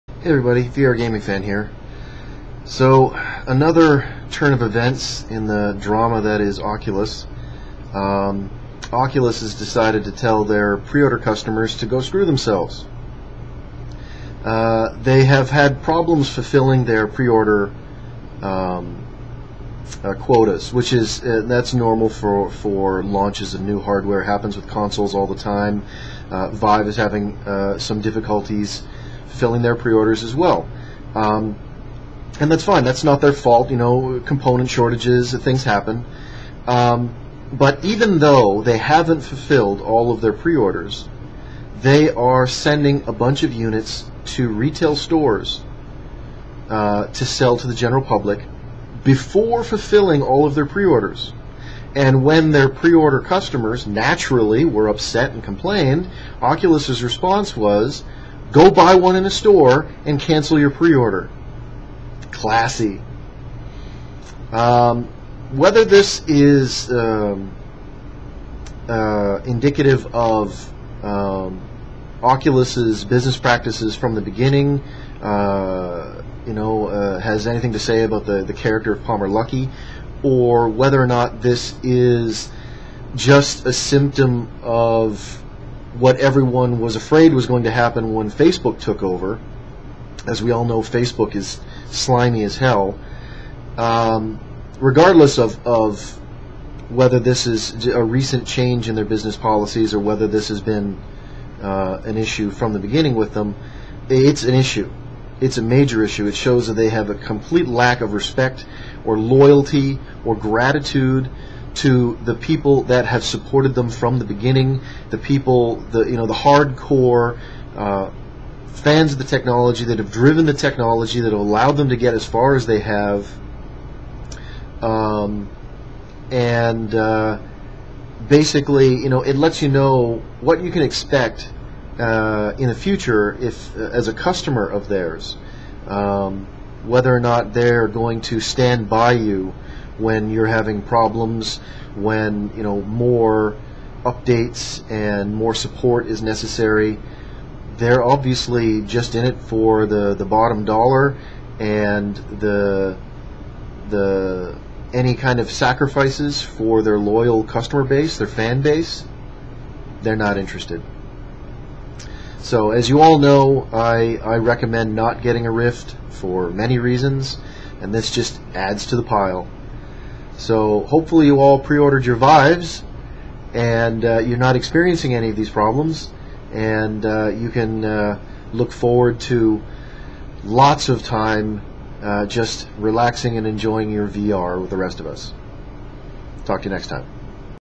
Vlog - Oculus tells pre-order customers go screw themselves
Really bad form Oculus, shame on you.